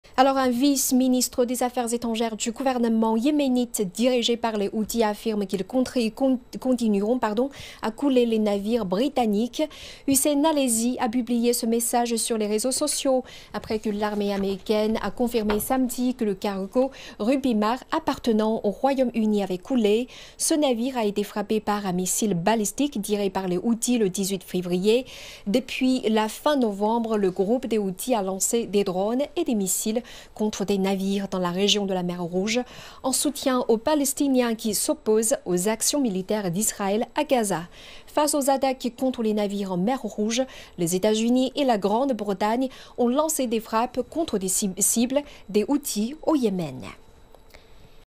Les Houthis du Yémen affirment qu'ils continueront à couler les navires britanniques
Un vice-ministre des Affaires étrangères du gouvernement yéménite dirigé par les Houthis affirme qu'ils continueront à couler les navires britanniques. Hussein al-Ezzi a publié ce message sur les médias sociaux, après que l'armée américaine a confirmé samedi que le cargo Rubymar, appartenant au Royaume-Uni, avait coulé. Ce navire a été frappé par un missile balistique tiré par les Houthis le 18 février. Depuis la mi-novembre, le groupe des Houthis a lancé des drones et des missiles contre des navires dans la région de la mer Rouge, en soutien aux Palestiniens qui s'opposent aux actions militaires d'Israël à Gaza. Face aux attaques contre les navires en mer Rouge, les États-Unis et la Grande-Bretagne ont lancé des frappes contre des cibles des Houthis au Yémen.